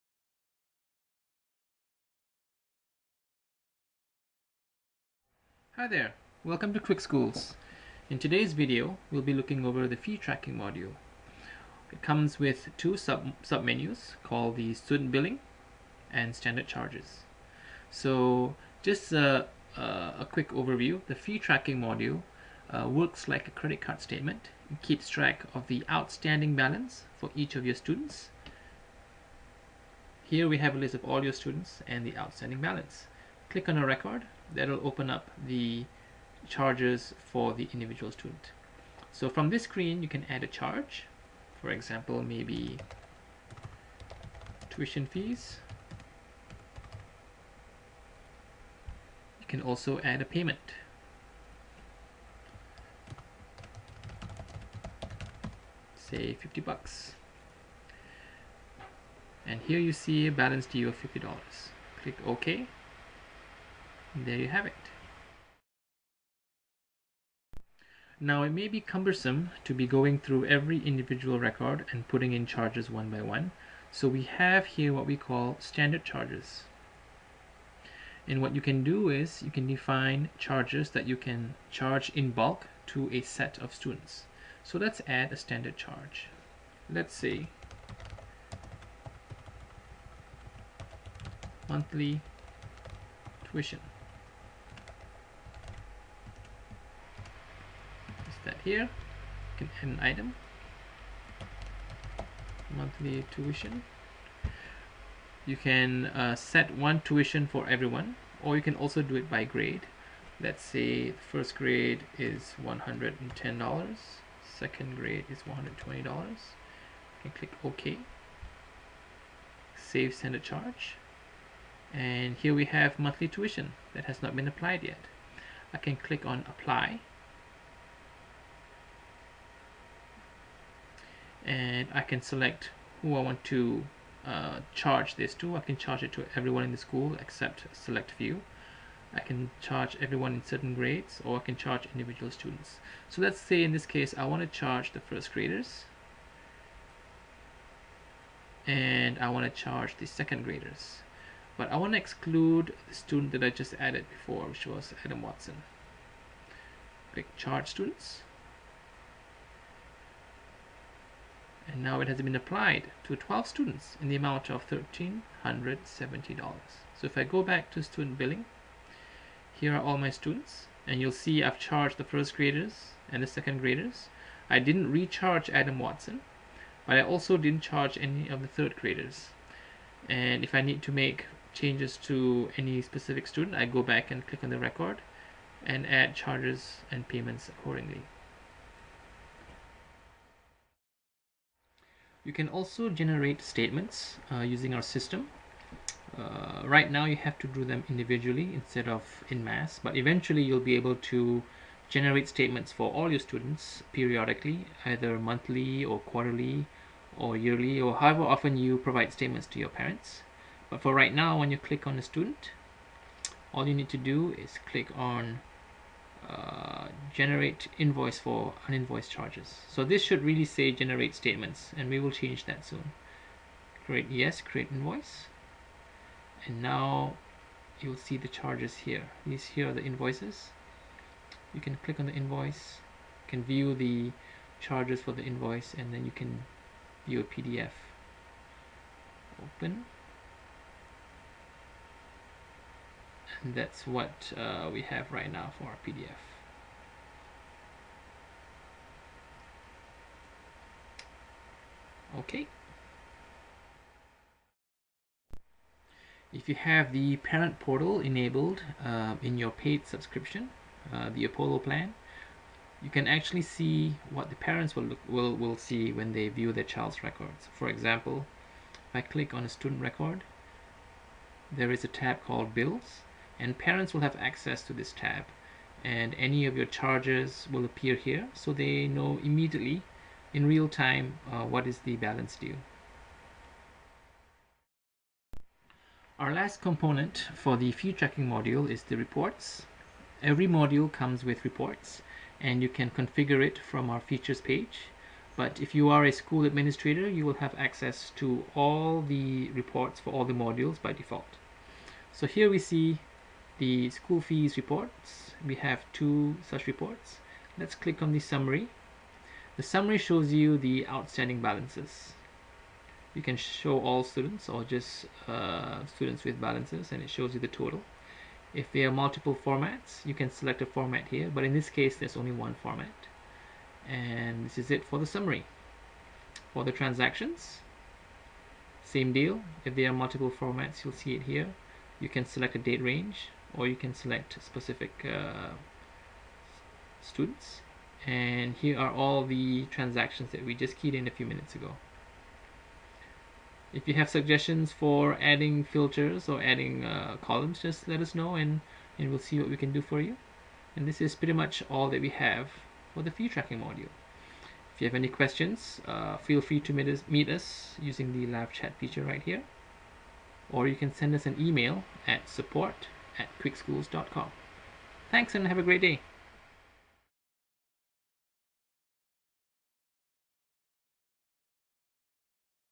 QuickSchools Fee Tracking Overview
This video shows how to use the Fee Tracking module on QuickSchools. Track fees, payments and outstanding balances for each of your students online. Sign up for a free 30-day trial